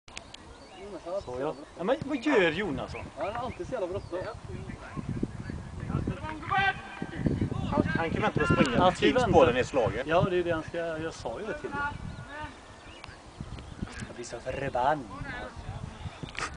Kabel-tv: Hörna i Rävlanda
FC Kabel Åttio spelar seriepremiär 2011 på Bråtaredsvallen i Rävlanda. Matchen slutar 2-1 till Rävlanda AIS. Klippet är från matchens inledning och de bruna brassarna har hörna från höger.